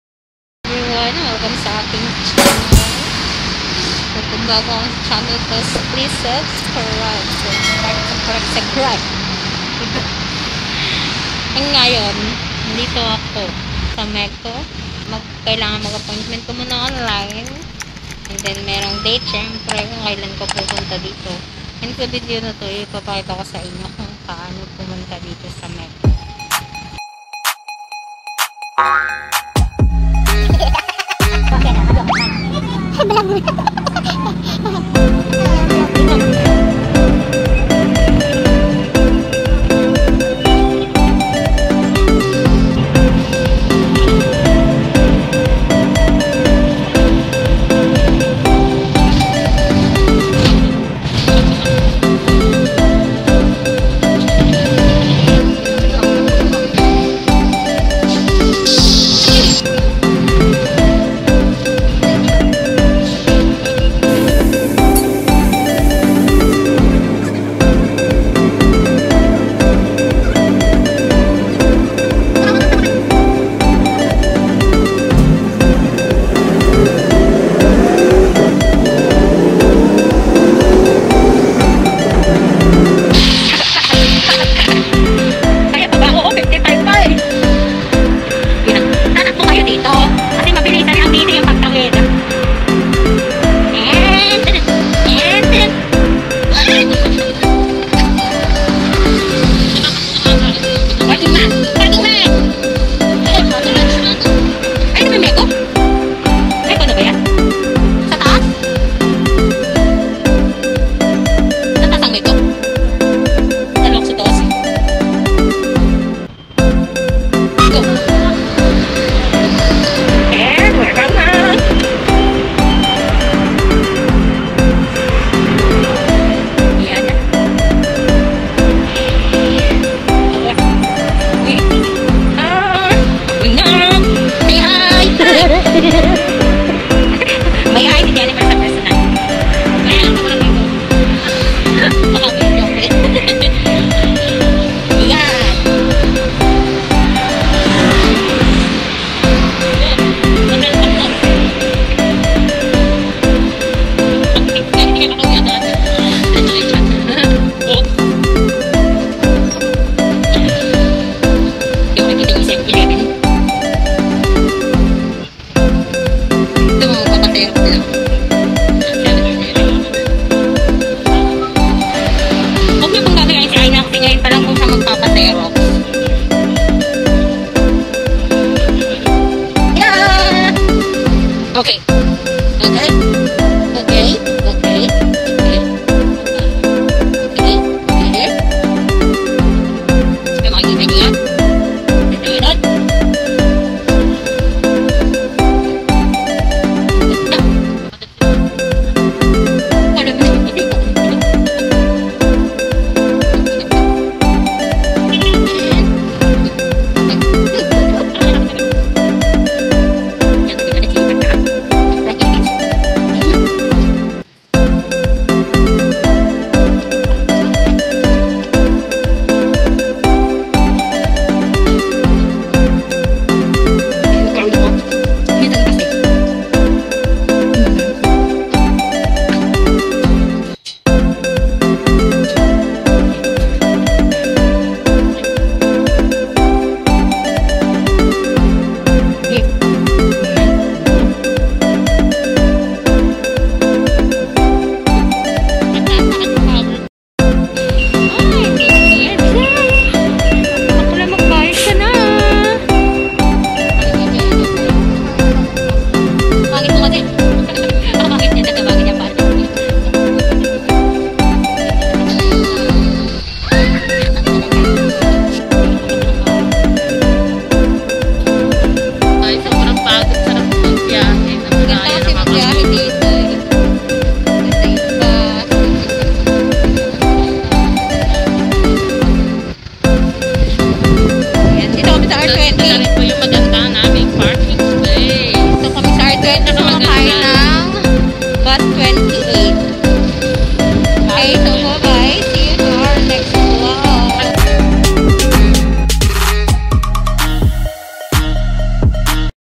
PAANO PUMUNTA SA MECO | Cheng's Vlog #6
In Taiwan Kaohsiung Only..

HOW TO GO TO MECO KAOHSIUNG (Manila Economic and Cultural Office)
Take a Bus #28 From R-20
R-20 po kase Diyan po kase kame galing,
Pasensya na, Kung First time mo..
First time ko rin po :)

I'm With Friends kaya Bida-Bida na rin ;)
Kaya naisipan kong Gumawa ng Video,
How to get there, Para Malaman niyo rin :D
Pero mas Marunong ang NagTatanong ;)

Here is the Link of MECO
If need niyo po mag pa-Appointment
To Re-New your Passport.